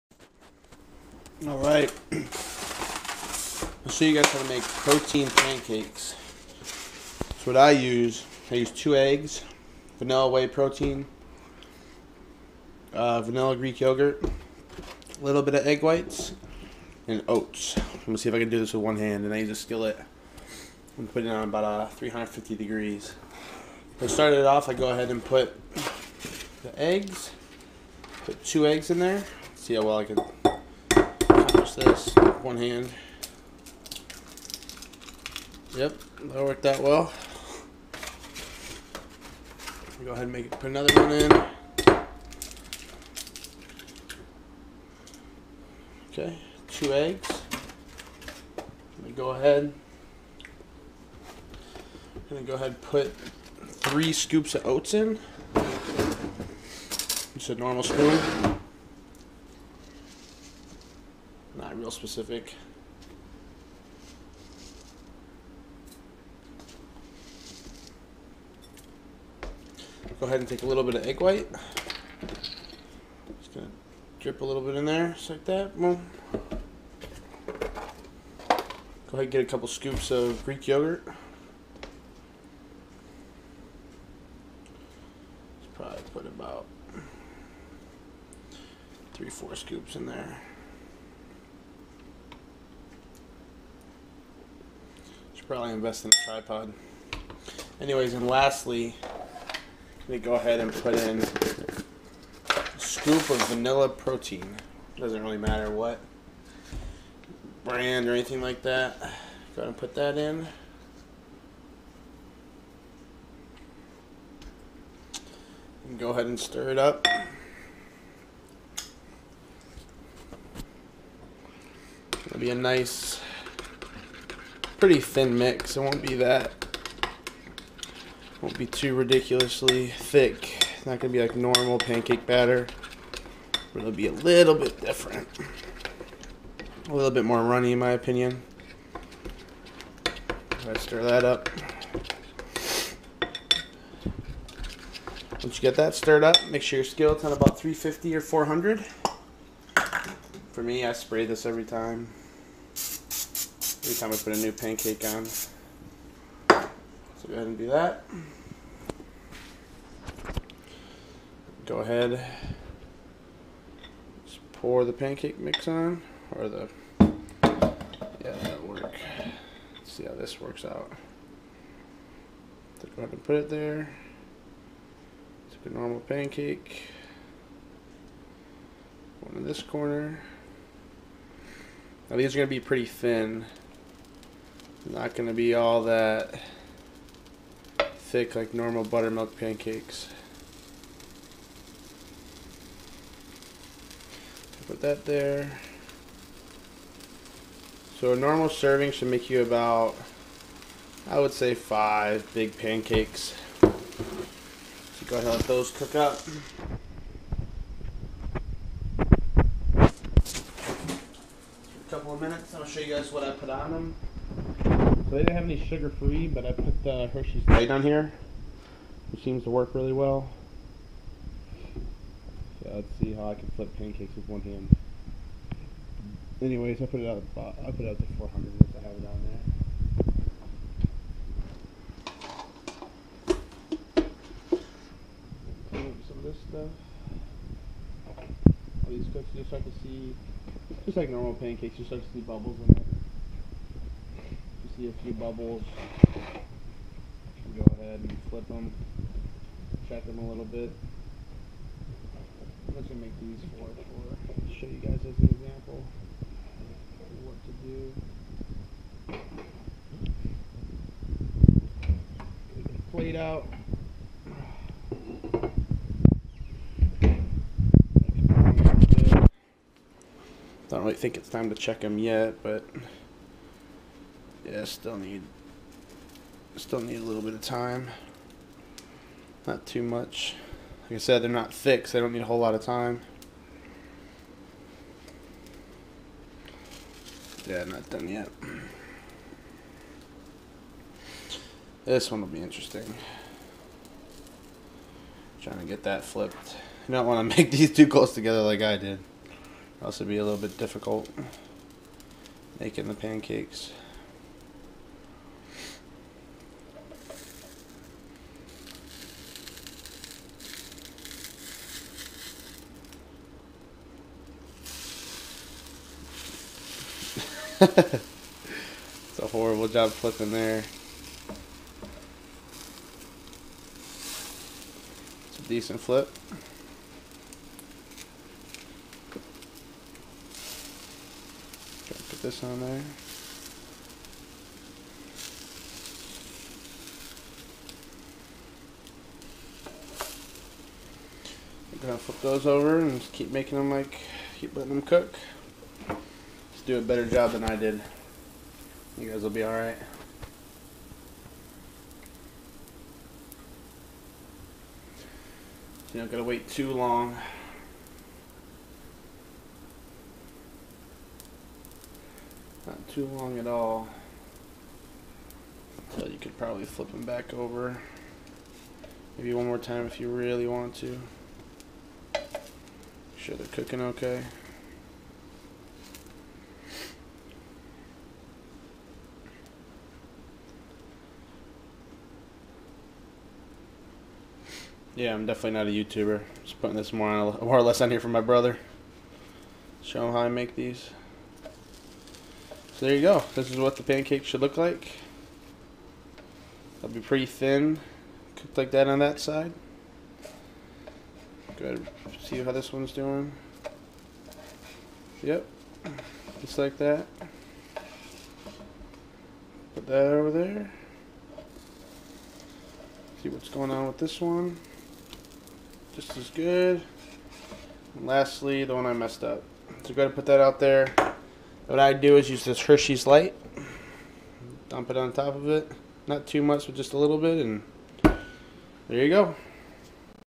Protein Pancakes! How To Video and Recipe!
Link to my #1 Amazon Best Selling eBook!

*2 Eggs

*3 Scoops of Oats

*3 Scoops of Vanilla Greek Yogurt

*1 Spoonful of Egg Whites

*1 Scoop of Vanilla Whey Protein